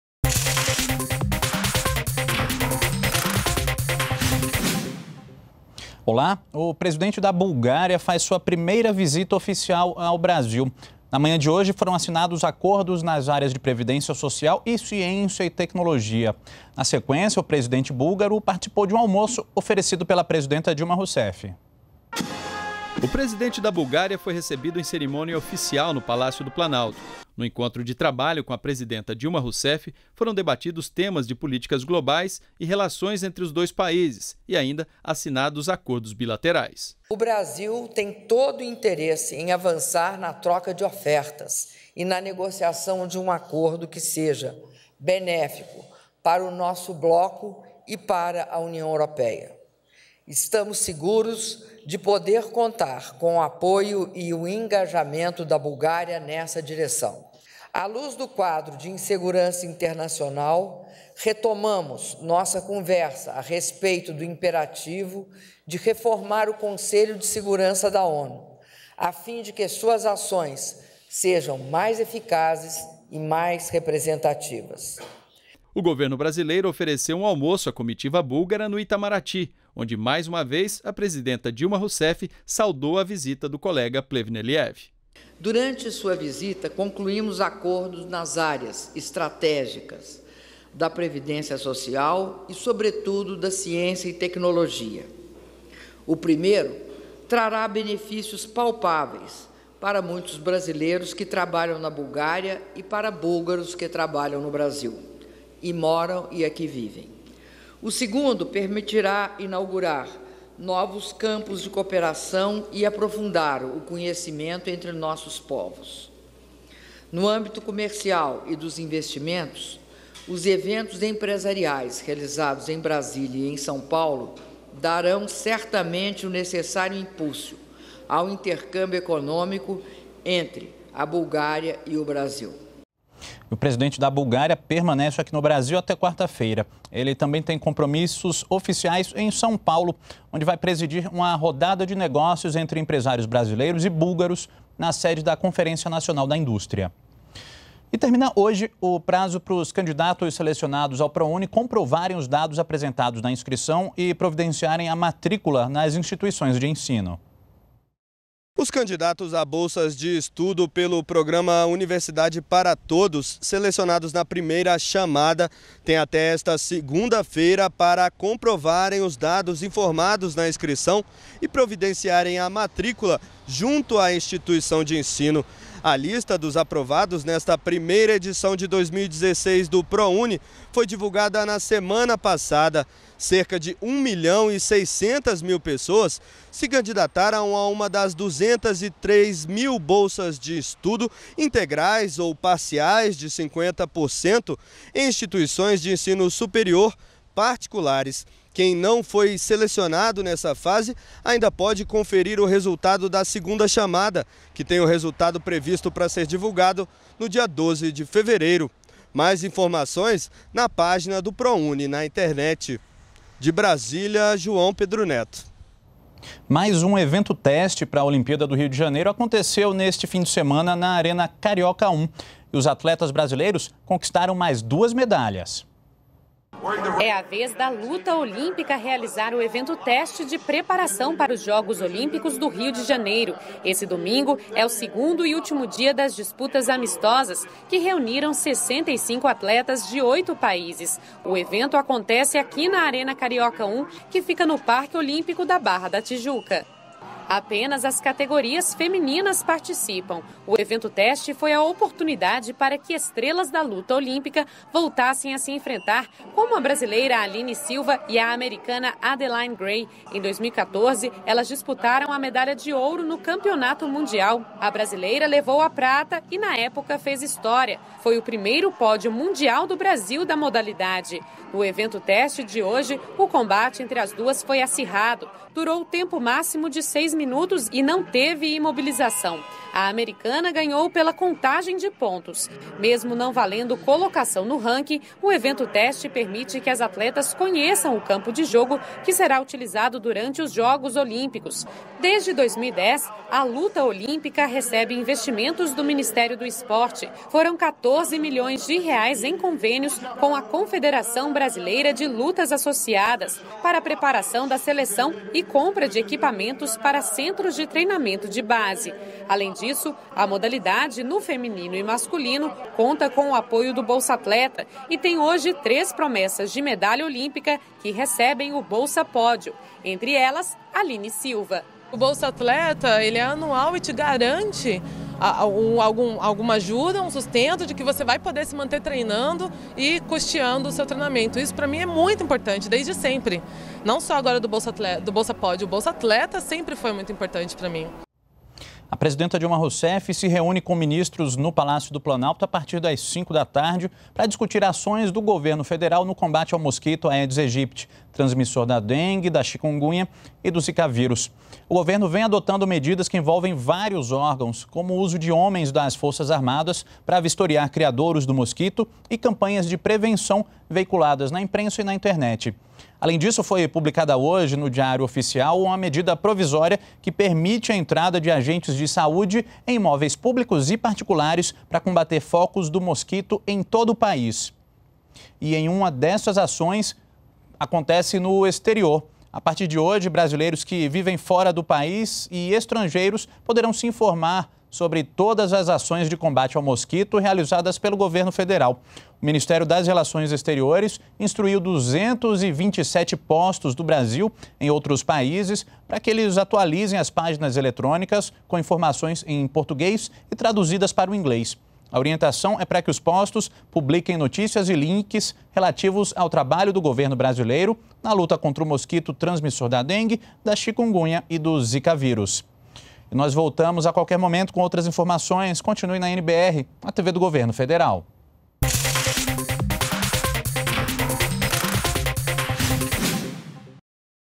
Últimas - Dilma Rousseff destaca importância de acordos firmados entre Brasil e Bulgária
ÚLTIMAS 16h: A presidenta Dilma Rousseff recebeu, nesta segunda-feira (1º), o presidente búlgaro Rosen Plevneliev, que realiza sua primeira visita oficial ao Brasil. Após assinatura de acordos bilaterais, a presidenta ofereceu um almoço à comitiva da Bulgária. Em discurso, Dilma destacou a importância dos acordos celebrados nas áreas científica, tecnológica e de Previdência Social. Plevneliev também visitará o estado de São Paulo, onde irá presidir uma rodada de negócios entre empresários brasileiros e búlgaros. Veja ainda: termina hoje o prazo para que candidatos aprovados no Prouni comprovem dados apresentados na inscrição e efetuem a matrícula nas instituições de ensino.